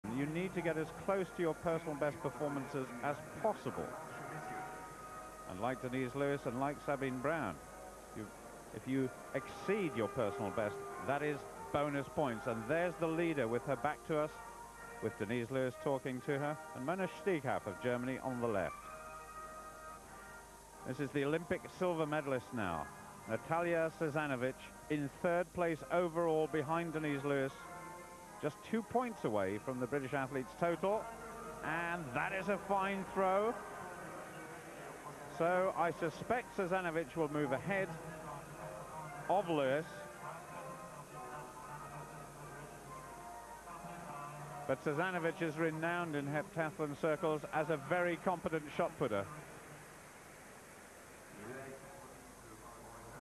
7150 World Track and Field 1997 Heptathlon Shot Put Natalya Sazanovich
1997 World Athens Heptathlon Shot Put Natalya Sazanovich NT
Buying something on Amazon
1 Sabine Braun (GER) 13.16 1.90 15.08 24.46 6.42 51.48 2:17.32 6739
2 Denise Lewis (GBR) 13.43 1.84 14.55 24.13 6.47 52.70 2:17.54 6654
3 Remigija Nazarovienė (LTU) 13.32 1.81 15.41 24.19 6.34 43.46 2:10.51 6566
4  Urszula Włodarczyk (POL) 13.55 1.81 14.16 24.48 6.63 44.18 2:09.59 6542
5  Natalya Sazanovich (BLR) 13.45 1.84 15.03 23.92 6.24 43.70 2:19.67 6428
6  Mona Steigauf (GER) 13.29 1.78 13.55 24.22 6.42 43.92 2:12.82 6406
7  LeShundra Nathan (USA) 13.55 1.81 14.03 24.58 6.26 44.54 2:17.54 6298
8  Irina Vostrikova (RUS) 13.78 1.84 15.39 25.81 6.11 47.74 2:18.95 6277
9  Peggy Beer (GER) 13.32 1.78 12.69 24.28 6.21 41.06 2:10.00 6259
10  Kelly Blair-LaBounty (USA) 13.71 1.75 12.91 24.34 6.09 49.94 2:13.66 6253
11  Nathalie Teppe (FRA) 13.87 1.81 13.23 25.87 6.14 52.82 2:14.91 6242
12  Marie Collonville (FRA) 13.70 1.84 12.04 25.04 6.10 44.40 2:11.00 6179
13  Tatyana Gordeyeva (RUS) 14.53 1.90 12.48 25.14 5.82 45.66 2:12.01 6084
14  Tiia Hautala (FIN) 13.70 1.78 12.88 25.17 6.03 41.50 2:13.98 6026
15  Joanne Henry (NZL) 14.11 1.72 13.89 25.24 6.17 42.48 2:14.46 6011
16  Imma Clopés (ESP) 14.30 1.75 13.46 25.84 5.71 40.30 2:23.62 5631
—  Svetlana Moskalets (RUS) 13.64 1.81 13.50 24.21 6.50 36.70 — DNF
—  Ma Chun-Ping (TPE) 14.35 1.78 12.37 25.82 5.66 — — DNF
—  Eunice Barber (SLE) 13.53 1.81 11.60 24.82 — — — DNF
—  Svetlana Kazanina (KAZ) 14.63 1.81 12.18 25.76 — — — DNF
—  Kym Carter (USA) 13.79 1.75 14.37 DNF — — — DNF
—  Athina Papasotiriou (GRE) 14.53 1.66 — — — — — DNF